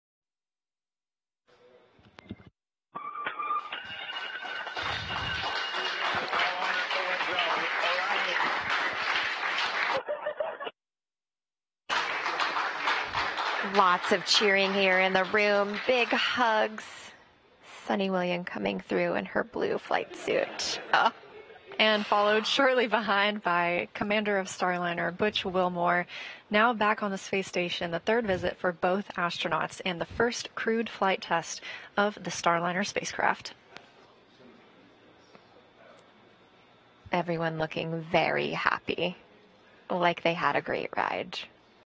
Indian-Origin Astronaut Sunita Williams (59) dances on her arrival at the International Space S
Indian-Origin Astronaut Sunita Williams (59) dances on her arrival at the International Space Station for her third trip.

The 58-year-old Astronaut has become the first woman to pilot and test a new crewed spacecraft on its maiden mission.